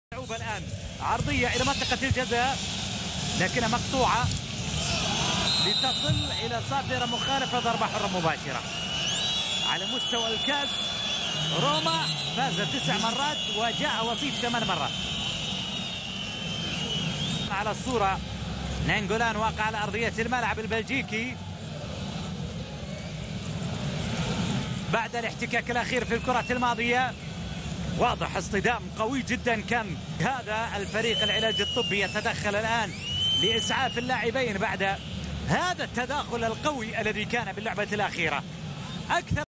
Nainggolan broke Federico Mattiello leg Chievo vs AS Roma 2015
DURA ENTRADA DE NAINGOLAN .